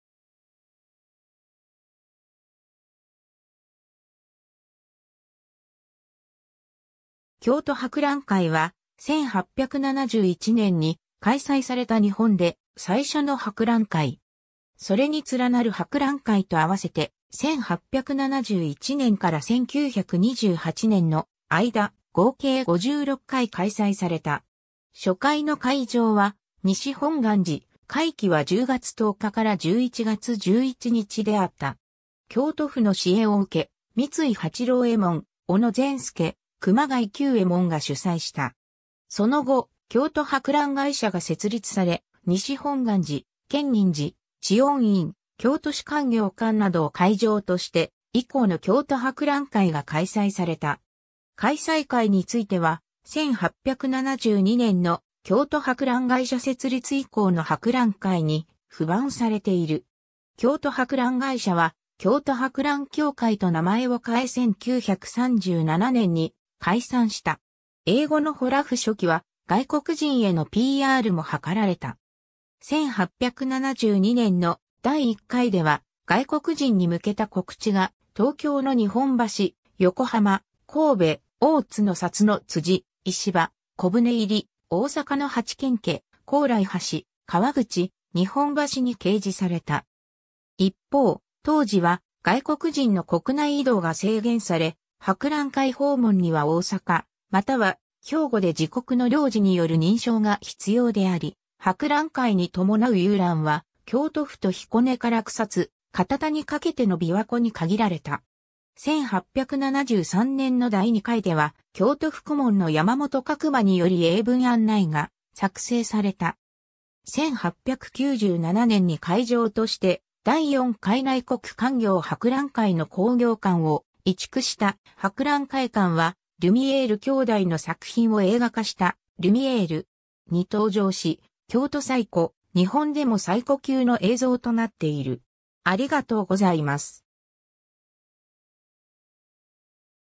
京都博覧会
京都府の行事・イベント
京都博覧会（きょうとはくらんかい）は、1871年（明治4年）に開催された日本で最初の博覧会。
 それに連なる博覧会と合わせて、1871年から1928年（昭和3年）の間、合計56回開催された。
初回の会場は西本願寺、会期は10月10日～11月11日であった。
京都府の支援を受け、三井八郎右衛門（三井財閥）・小野善助（小野組）・熊谷久右衛門（鳩居堂）が主催した。
その後、京都博覧会社が設立され、西本願寺・建仁寺・知恩院・京都市勧業館などを会場として、以降の京都博覧会が開催された。
開催回については、1872年の京都博覧会社設立以降の博覧会に付番されている。
京都博覧会社は京都博覧協会と名前を変え1937年に解散した。
 英語のホラフ（第1回） 初期は外国人へのPRも図られた。
1872年の第1回では外国人に向けた告知が、東京の日本橋、横浜、神戸、大津の札の辻・石場・小舟入、大阪の八軒家・高麗橋・川口・日本橋に掲示された。
一方、当時は外国人の国内移動が制限され、博覧会訪問には大阪または兵庫で自国の領事による認証が必要であり、博覧会に伴う遊覧は京都府と彦根から草津・堅田にかけての琵琶湖（滋賀県）に限られた。
1873年の第2回では京都府顧問の山本覚馬により英文案内が作成された。
 1897年に会場として第4回内国勧業博覧会の工業館を移築した博覧会館は、リュミエール兄弟の作品を映画化した「リュミエール！」に登場し、京都最古、日本でも最古級の映像となっている。